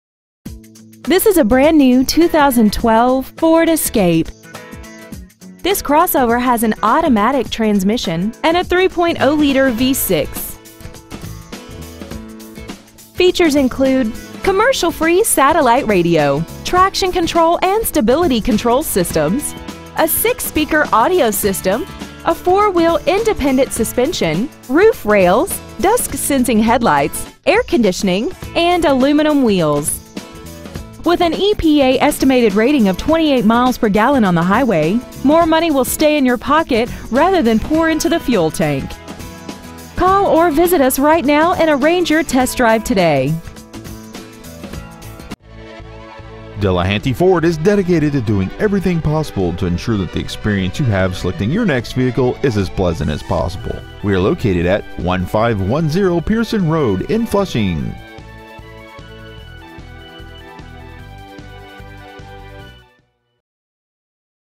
2012 Ford Escape Flushing MI
Year : 2012
 Make : Ford
 Model : Escape
 Engine : 3.0L V6 24V MPFI DOHC FLEXIBLE FUEL
 Trans . : 6-SPEED AUTOMATIC
 Exterior : EBONY BLACK

 Interior : CHARCOAL BLACK
 Stock : F20427